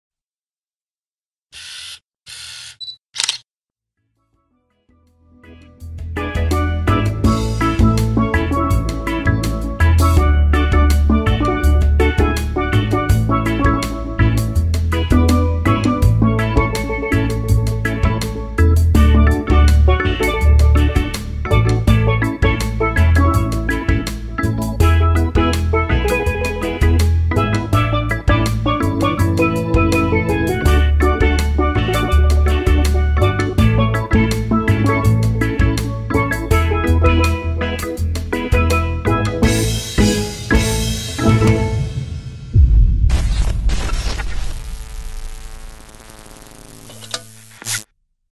Camaralia te enseña: Panasonic AG-DVX200
En este vídeo os mostramos una presentación de una de las últimas maravillas que ha sacado a la venta Panasonic. La Panasonic AG-DVX200 ofrece imágenes impresionantes gracias al potente objetivo LEICA DICOMAR 4K de 13 aumentos, combinado con un sensor 4K de gran formato y un conjunto de funciones estéticas, como la reducción de profundidad de campo, un Bokeh atractivo y un increíble rango dinámico de 12 pasos, gracias a la adopción de la curva V-Log L de VariCam.